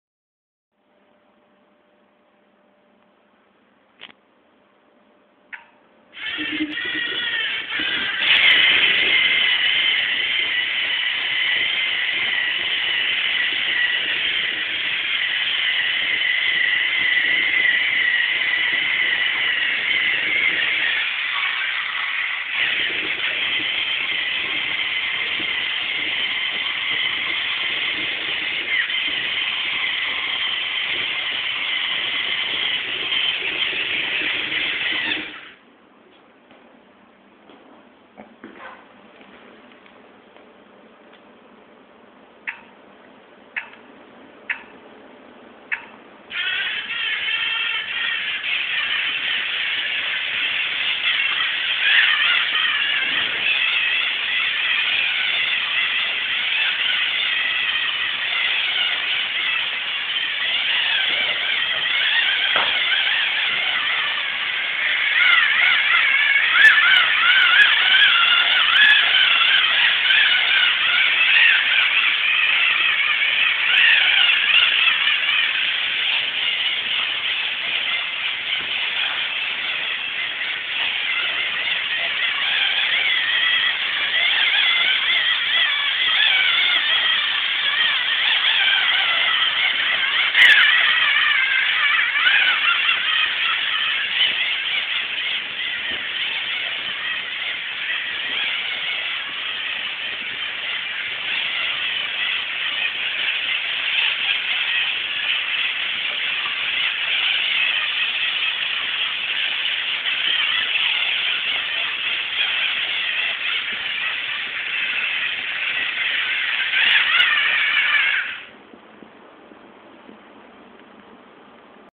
Line Following Robot (Lego Mindstorms NXT 2.0)
Everyone must build their own line following robot. Here's mine.